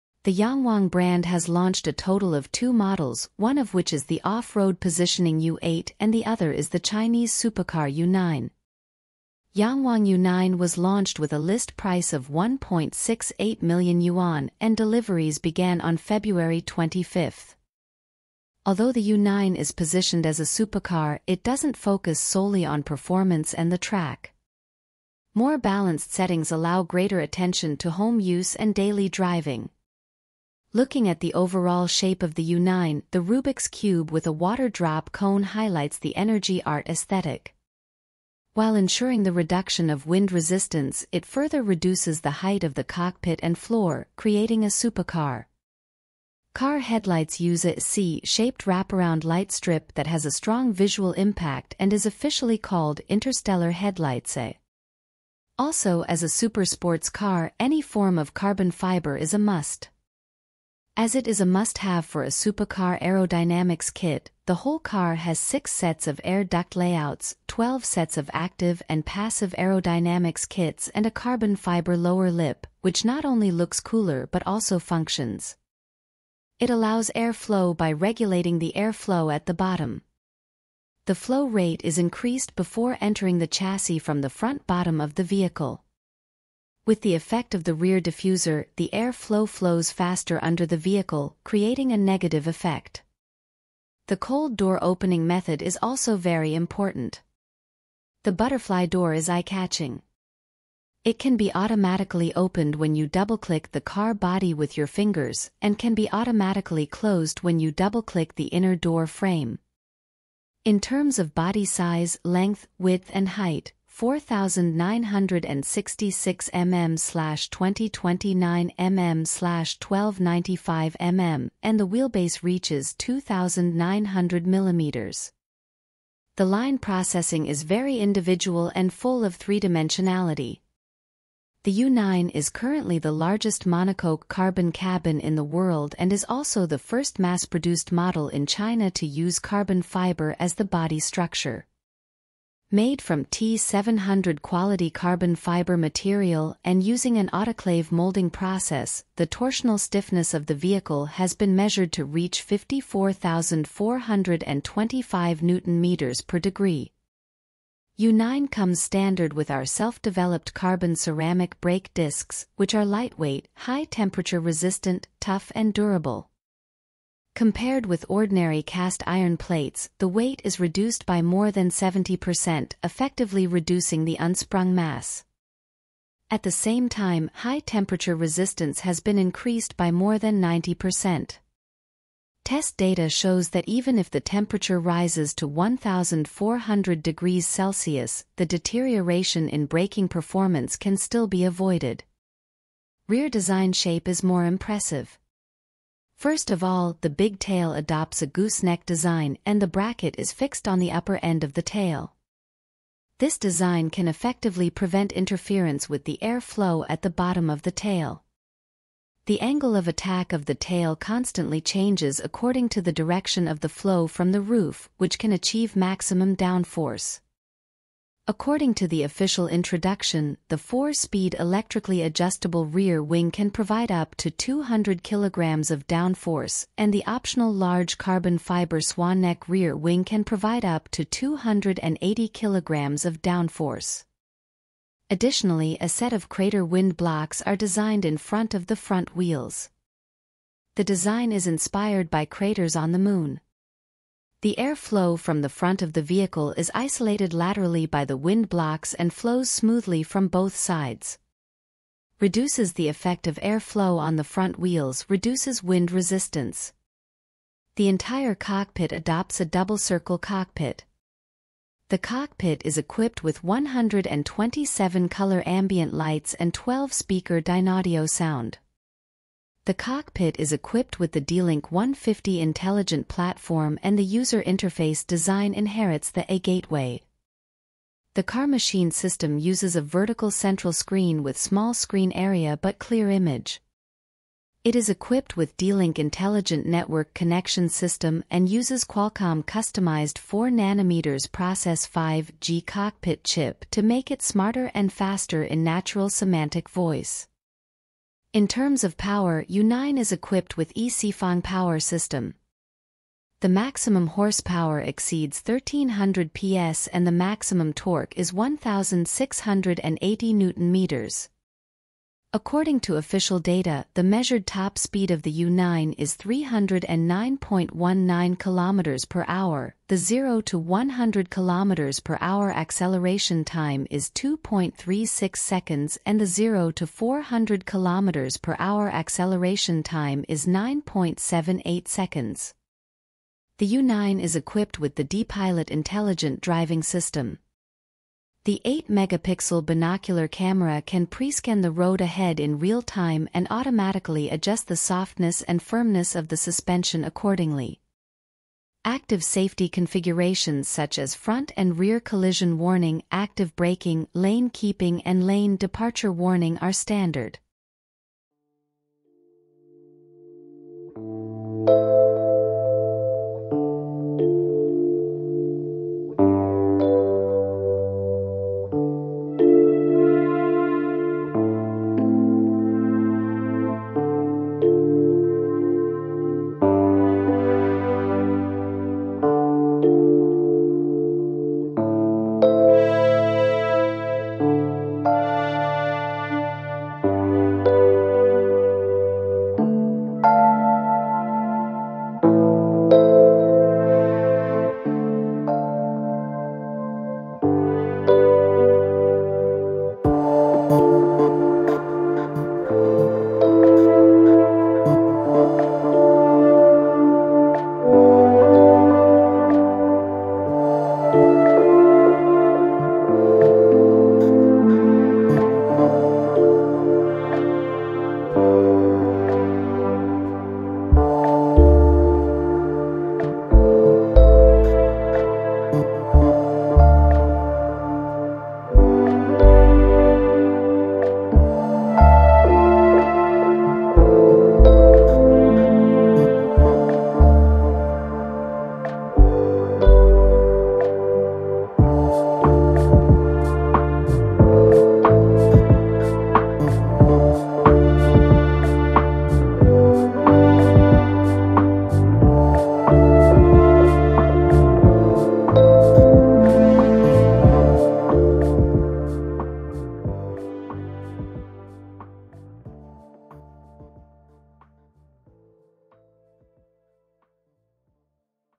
Horsepower Exceeding 1300Ps, Maximum Speed 310 Km, 0-100 Speed 2.36 Seconds New Byd Yangwang U9 2024
The Yangwang brand has launched a total of two models, one of which is the off-road positioning U8 and the other is the Chinese supercar U9. Yangwang U9 was launched with a list price of 1.68 million yuan, and deliveries began on February 25.

Although the U9 is positioned as a supercar, it doesn't focus solely on performance and the track. More balanced settings allow greater attention to home use and daily driving.

Looking at the overall shape of the U9, the Rubik's Cube with a water drop cone highlights the energy art aesthetic. While ensuring the reduction of wind resistance, it further reduces the height of the cockpit and floor, creating a supercar.

Car headlights use a "C"-shaped wraparound light strip that has a strong visual impact and is officially called "Interstellar Headlights". Also, as a super sports car, any form of carbon fiber is a must.

As it is a must-have for a supercar aerodynamics kit, the whole car has 6 sets of air duct layouts, 12 sets of active and passive aerodynamics kits and a carbon fiber lower lip, which not only looks cooler but also functions. It allows air flow by regulating the air flow at the bottom. The flow rate is increased before entering the chassis from the front bottom of the vehicle. With the effect of the rear diffuser, the air flow flows faster under the vehicle, creating a negative effect.

The cold door opening method is also very important. The butterfly door is eye-catching. It can be automatically opened when you double-click the car body with your fingers, and can be automatically closed when you double-click the inner door frame.

In terms of body size, length, width and height: 4966 mm/2029 mm/1295 mm, and the wheelbase reaches 2900 mm. The line processing is very individual and full of three-dimensionality.

The U9 is currently the largest monocoque carbon cabin in the world and is also the first mass-produced model in China to use carbon fiber as the body structure. Made from T700 quality carbon fiber material and using an autoclave molding process, the torsional stiffness of the vehicle has been measured to reach 54425Nm/°.

U9 comes standard with our self-developed carbon ceramic brake discs, which are lightweight, high temperature resistant, tough and durable. Compared with ordinary cast iron plates, the weight is reduced by more than 70%, effectively reducing the unsprung mass. At the same time, high temperature resistance has been increased by more than 90%. Test data shows that even if the temperature rises to 1400°C, the deterioration in braking performance can still be avoided.

Rear Design shape is more impressive. First of all, the big tail adopts a goose neck design, and the bracket is fixed on the upper end of the tail. This design can effectively prevent interference with the air flow at the bottom of the tail. The angle of attack of the tail constantly changes according to the direction of the flow from the roof, which can achieve maximum downforce.

According to the official introduction, the 4-speed electrically adjustable rear wing can provide up to 200 kg of downforce, and the optional large carbon fiber swan-neck rear wing can provide up to 280 kg of downforce.

Additionally, a set of crater wind blocks are designed in front of the front wheels. The design is inspired by craters on the moon. The air flow from the front of the vehicle is isolated laterally by the wind blocks and flows smoothly from both sides. Reduces the effect of air flow on the front wheels, reduces wind resistance.

The entire cockpit adopts a double-circle cockpit. The cockpit is equipped with 127-colour ambient lights and 12-speaker Dynaudio sound. The cockpit is equipped with the DiLink150 intelligent platform and the user interface design inherits the "Gateway".

The car-machine system uses a vertical central screen with small screen area but clear image. It is equipped with DiLink intelligent network connection system and uses Qualcomm customized 4nm process 5G cockpit chip to make it smarter and faster in natural semantic voice.

In terms of power, U9 is equipped with Yi Sifang power system. The maximum horsepower exceeds 1300Ps and the maximum torque is 1680Nm. According to official data, the measured top speed of the U9 is 309.19 km/h, the 0-100 km/h acceleration time is 2.36 seconds and the 0-400 km/h acceleration time is 9.78 seconds.

The U9 is equipped with the DiPilot intelligent driving system. The 8-megapixel binocular camera can pre-scan the road ahead in real time and automatically adjust the softness and firmness of the suspension accordingly. Active safety configurations such as front and rear collision warning, active braking, lane keeping and lane departure warning are standard.